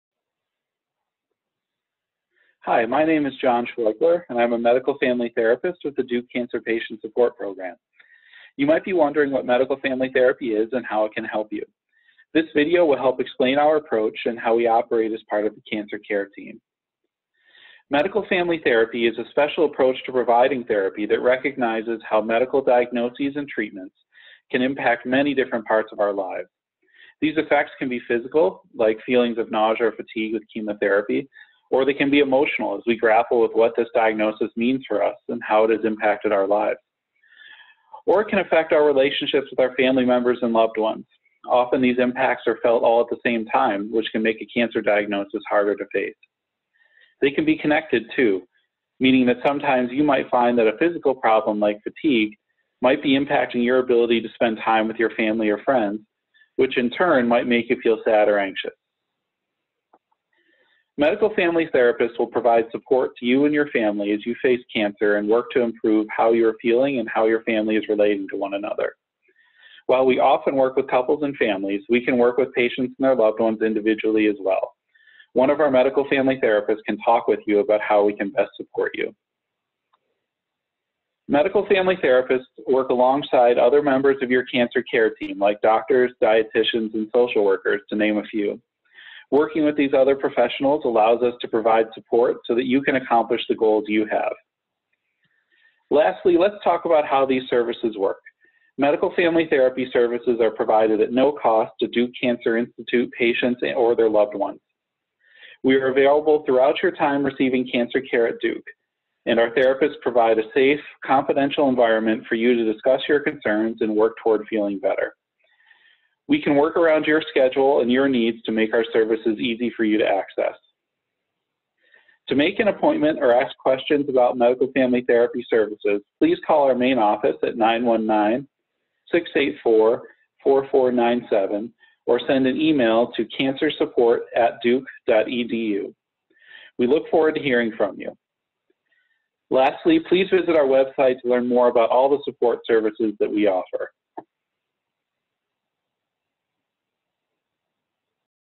Medical Family Therapy: What is it & is it for me
The Duke Cancer Patient Support Program's medical family therapists are available at no cost to support patients and their loved ones who are receiving cancer care at Duke Cancer Institute. Watch this video to learn more about what medical family therapy is and how it can help. Services are offered throughout the cancer experience—from diagnosis through treatment, recovery, and survivorship, as well as through the circumstances surrounding end of life.
To learn more about supportive care and survivorship services and resources available at Duke Cancer Institute visit, DukeCancerInstitute.org/SupportiveCare.